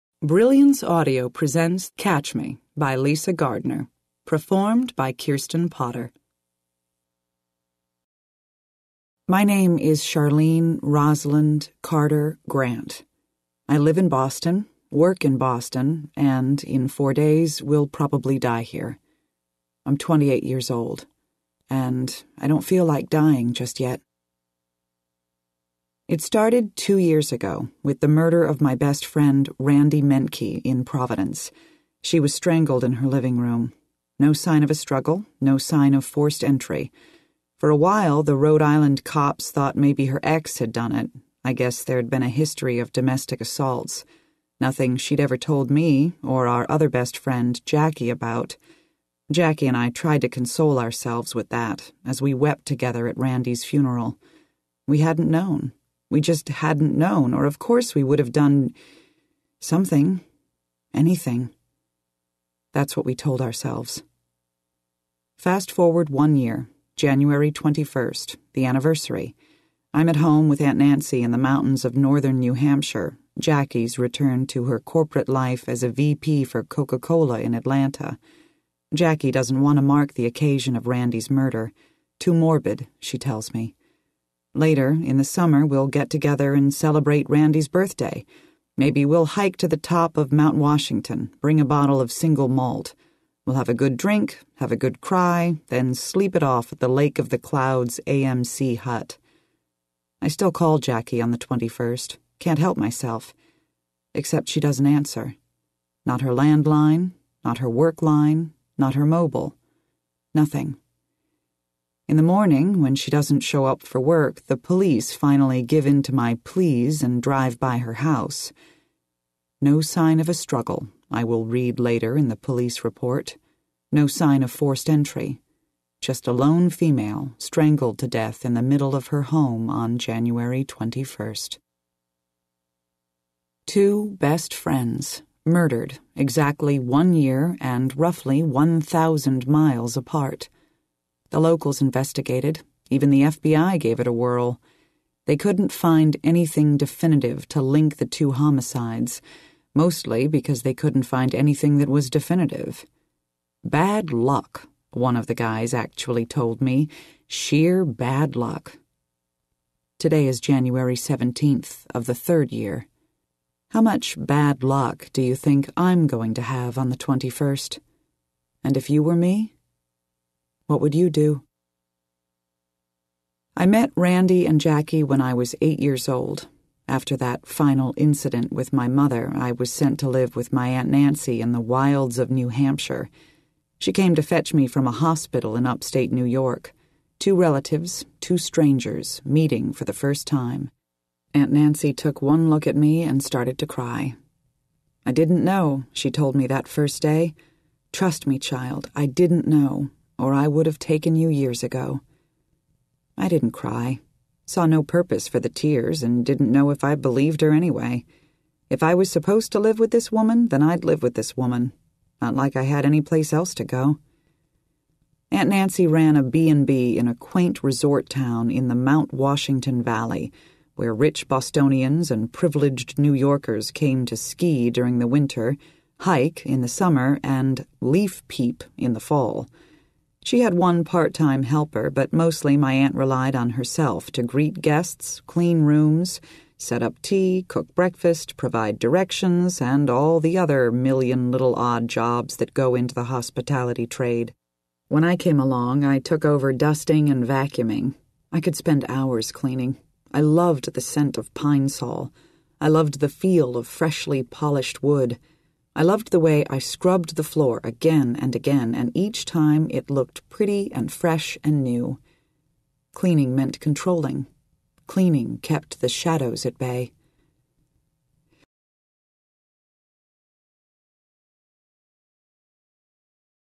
Catch Me by Lisa Gardner
Brilliance Audio presents Catch Me by Lisa Gardner, performed by Kirsten Potter.